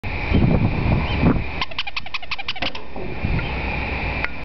Quinn Lewis's squeaky lobster as art doorbell
Quinn Lewis decorated his front door with lots of plastic animals, and sprayed them silver. The squeaky lobster is the doorbell.